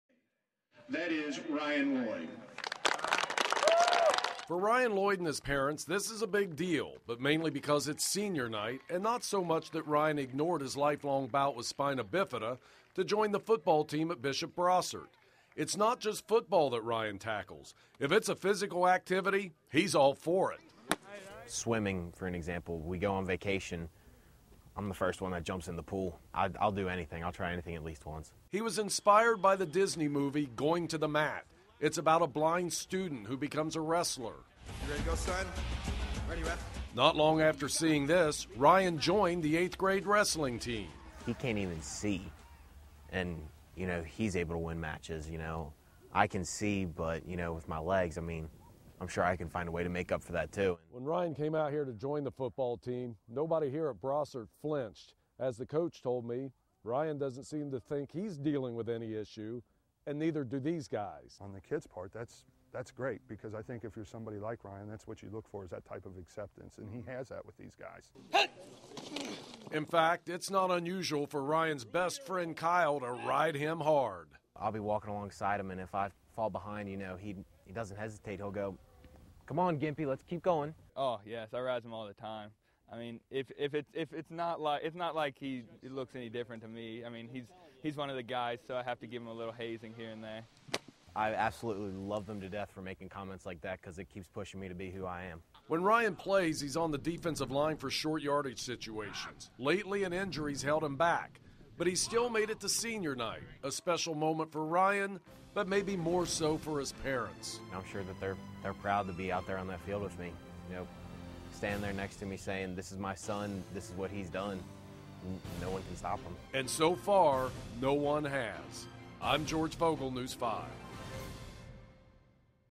Teen Overcomes Spina Bifida To Play Football
Ryan Lloyd is not your average high school football player.  He has spina bifida, but that hasn't stopped him from hitting the gridiron.  George Vogel reports.